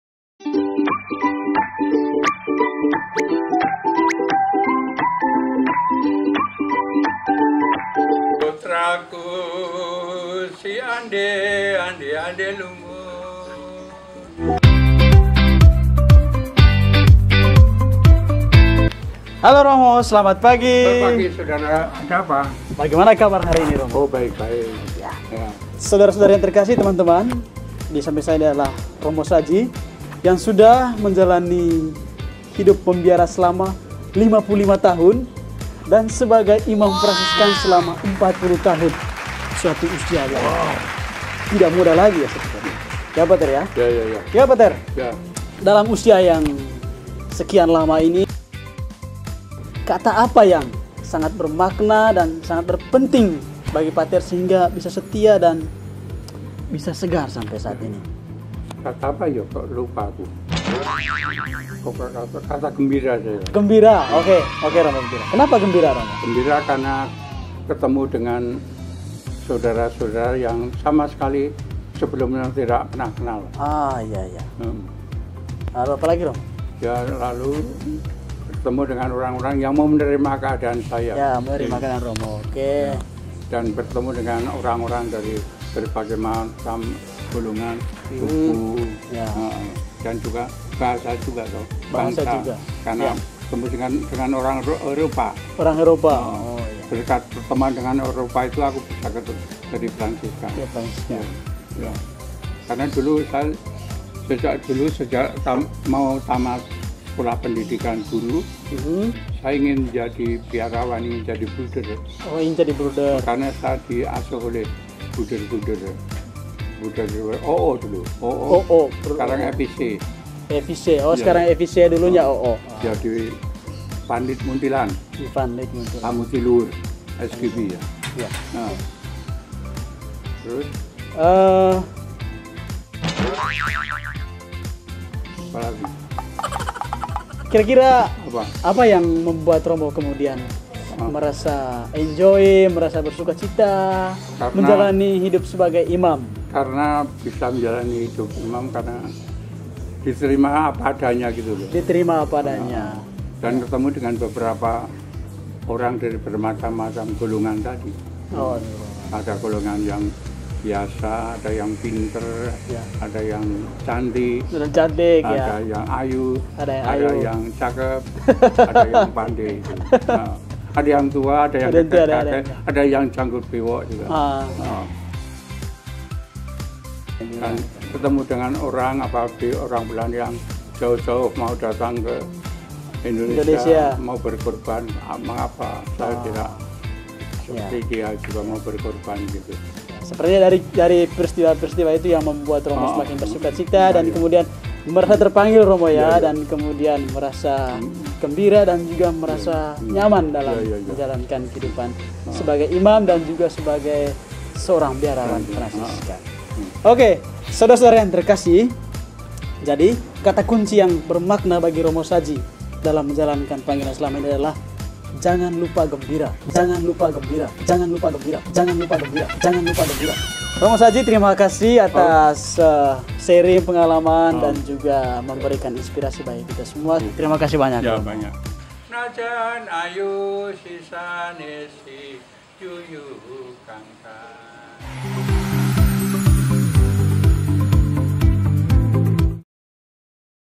Part 2 SATU KATA BERMAKNA dalam Panggilan Sdr. Bernardinus Sadji OFM
Merayakan Minggu Panggilan di tengah pandemi COVID 19, FransiskanMediaCentre (FMC) menyapa beberapa Saudara Fransiskan yang sudah lama menghayati panggilan mereka sebagai Fransiskan dan imam.
Kali ini, challenge FMC adalah: Sebutkan dan jelaskan makna SATU KATA yang amat bermakna dalam perjalanan hidup panggilan selama berpuluh-puluh tahun ini?

Edisi SATU KATA BERMAKNA  di hari Minggu Panggilan 2020 ini terdiri dari 4 part (video) masing-masing dari 4 saudara.

Semoga menginspirasi. Salam Sukacita!
pace e bene

FMC

***koreksi:
Sdr. Sadji OFM telah menghidupi imamatnya selama hampir 47 tahun. Beliau ditahbiskan September 1973 bersama P. Yustinus Semiun OFM & alm. P. D. Parto OFM.
Mohon maaf. Terima kasih. Tuhan memberkati.